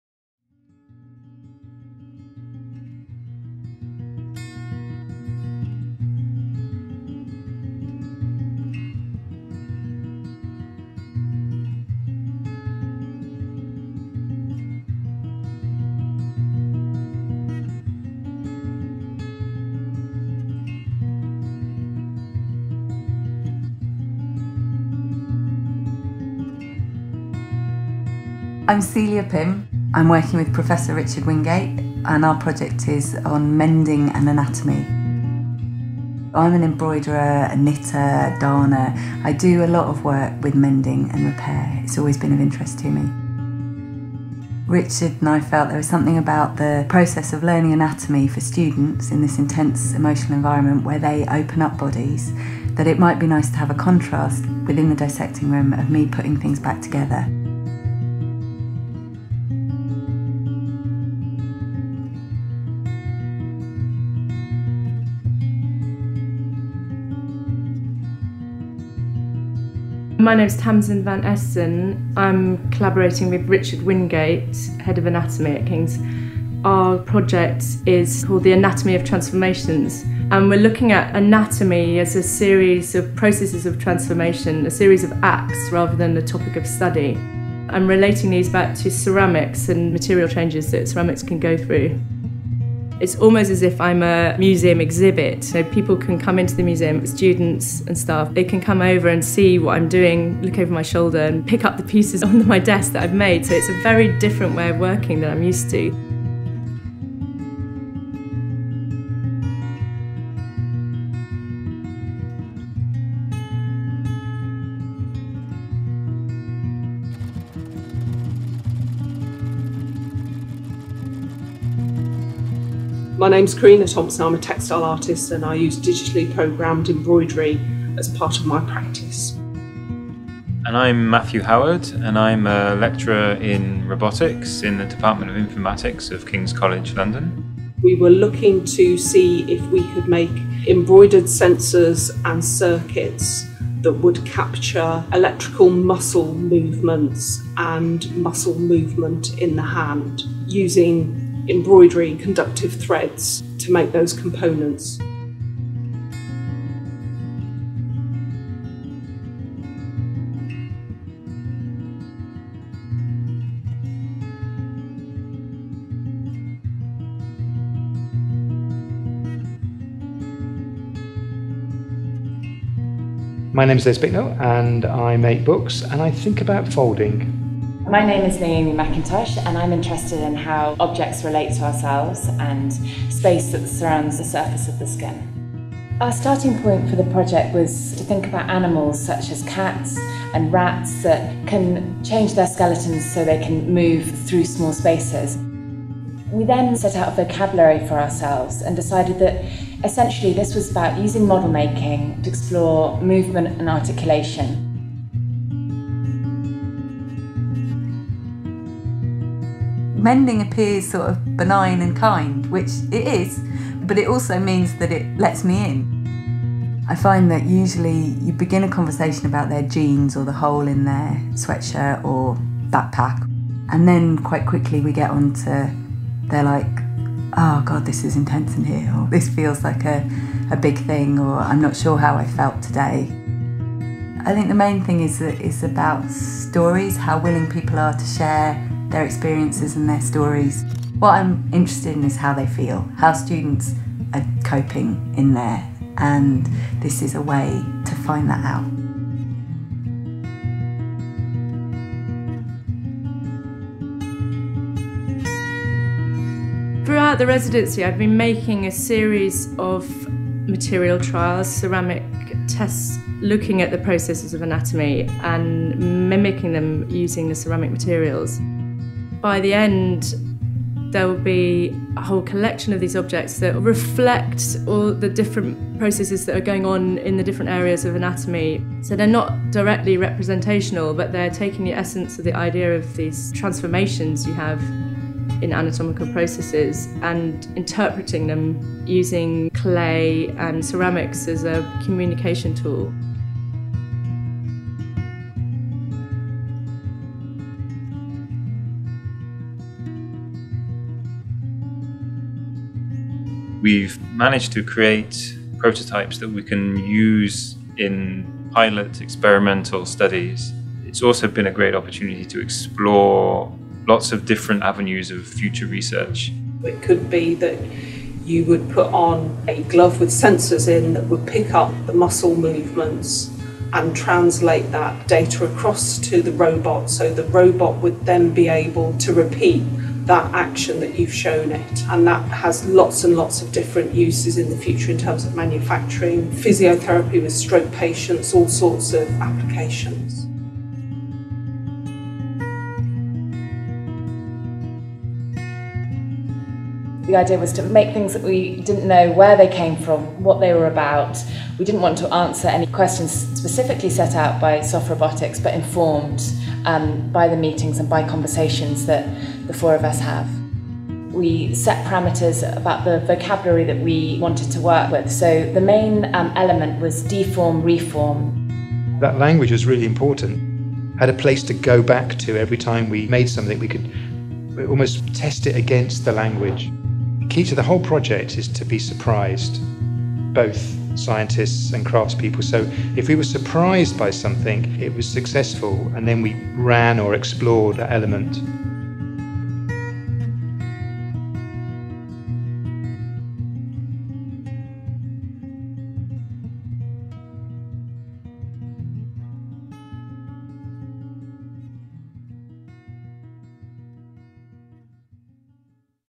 Parallel Practices 2014
The 2014 pilot project consisted of four collaborations, with each collaboration lasting four months and involving a team of at least one maker and one medical and scientific academic. These pairings stimulated learning and innovation through a focus on the body, materials and processes.